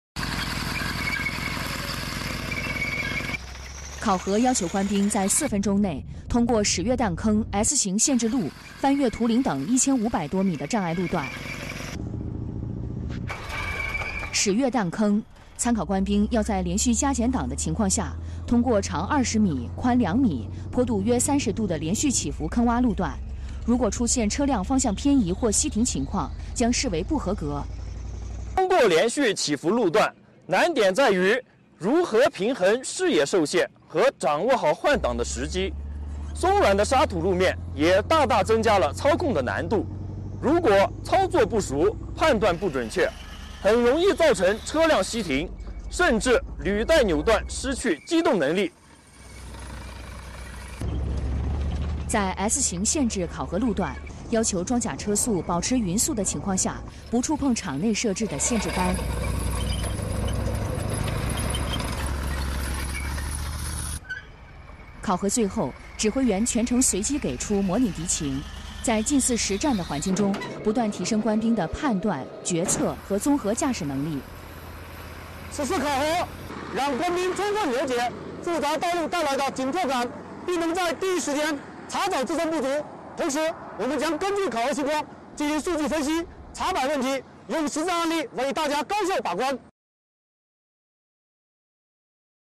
【直击演训场】海拔4000米 装甲车驾驶专业考核
近日，在海拔4000多米的高原训练场，西藏军区某合成旅组织装甲车驾驶专业技能考核，瞄准战场环境设置考核课目，着力锤炼全能型驾驶人才。
（军事报道·中国军号出品）